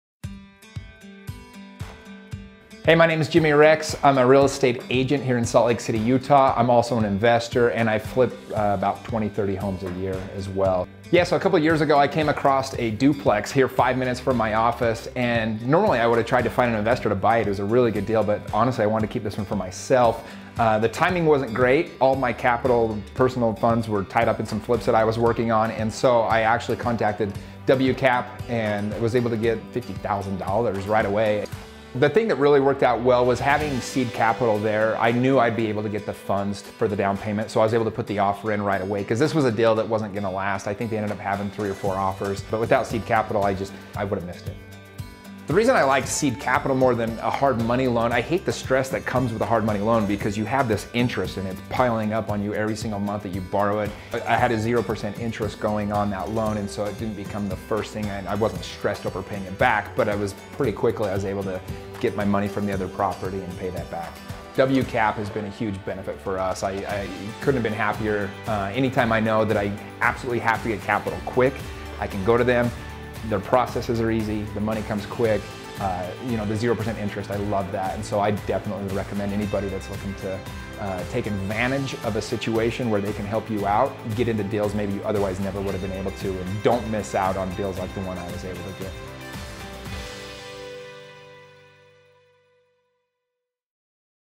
Raise $50,000 in Funding for Real Estate Investing
Jimmy is a real estate agent & investor who needed quick funding for a great deal.  He was able to raise $50k in unsecured seed capital with WCAP Financial for his deal within a few weeks.